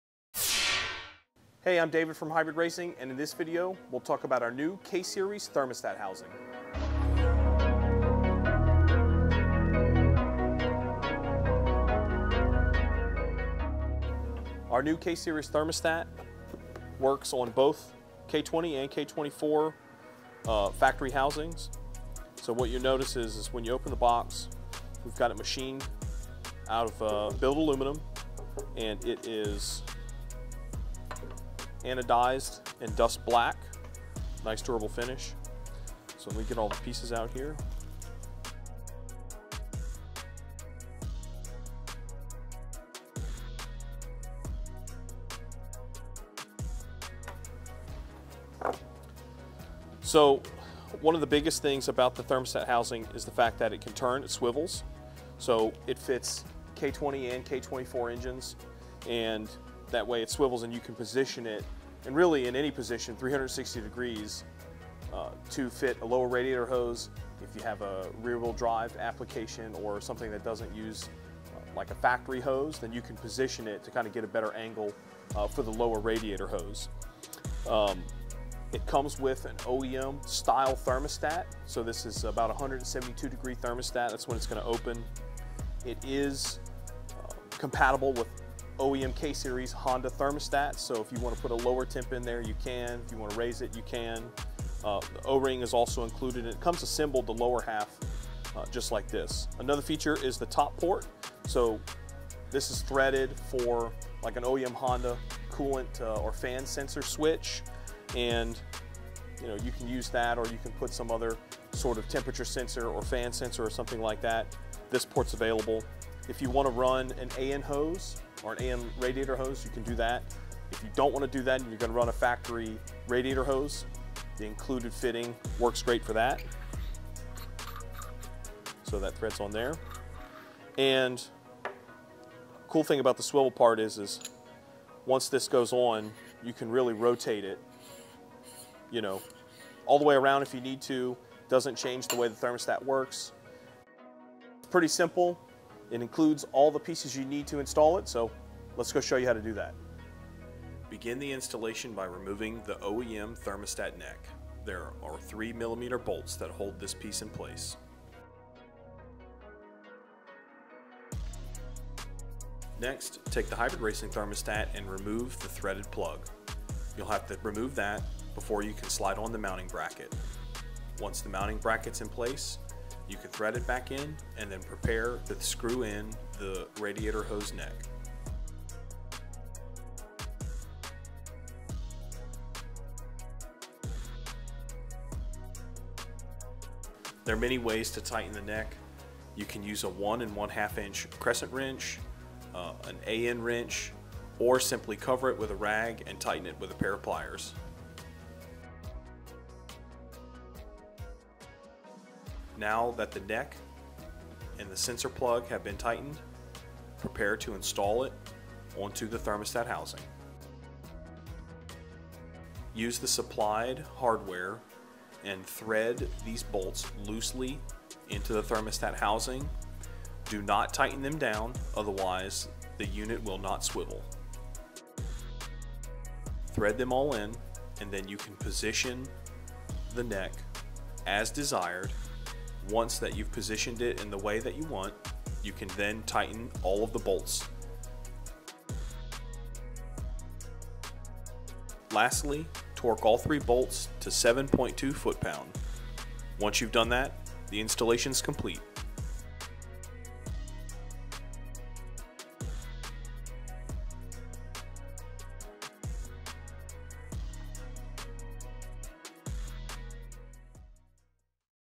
Hybrid Racing Adjustable K-Series Thermostat Housing
In this video, we go over a few of the points and features of our K-Series thermostat housing. This product was designed to be used with the most popular K-Series engines.

The housing can be rotated to accommodate different styles of radiator hoses and cooling situations. We include an extra sensor port, as well as a threaded hose neck that accepts OEM hoses or a -16ORB port if you choose to run AN lines.

These kits are machined from billet aluminum, anodized black and include an OEM K-Series thermostat (172*F) and gasket. Hardware in included and the installation is very simple.